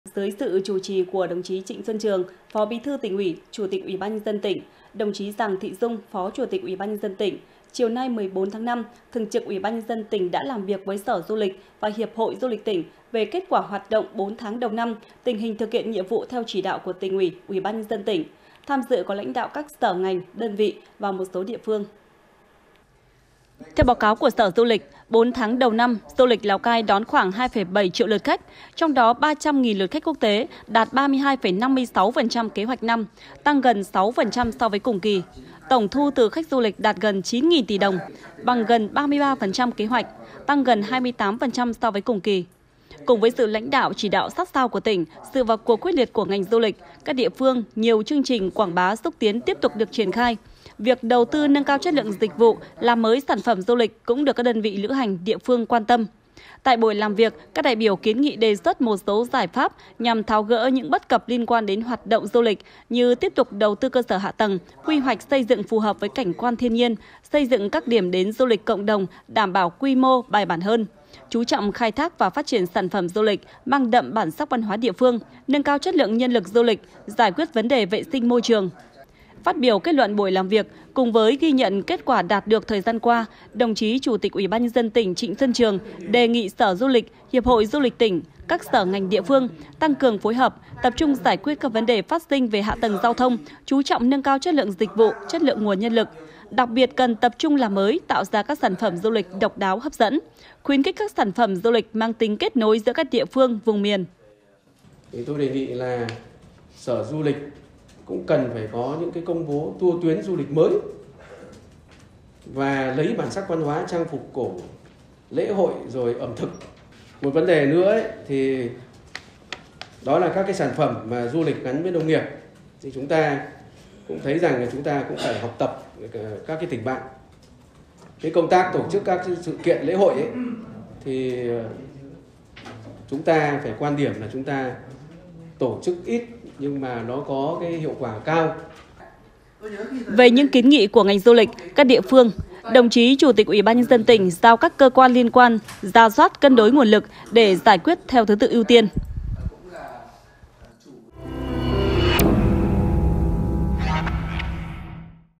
Thường trực UBND tỉnh Lào Cai làm việc với Sở Du lịch, Hiệp hội Du lịch tỉnh | THLC
Laocaitv.vn - Dưới sự chủ trì của đồng chí Trịnh Xuân Trường, Phó Bí thư Tỉnh ủy, Chủ tịch UBND tỉnh; đồng chí Giàng Thị Dung, Phó Chủ tịch UBND tỉnh, chiều 14/5, Thường trực UBND tỉnh đã làm việc với Sở Du lịch và Hiệp hội Du lịch tỉnh về kết quả hoạt động 4 tháng đầu năm; tình hình thực hiện nhiệm vụ theo chỉ đạo của Tỉnh ủy, UBND tỉnh. Tham dự có lãnh đạo các sở, ngành, đơn vị và một số địa phương.

Đài Phát thanh - Truyền hình Lào Cai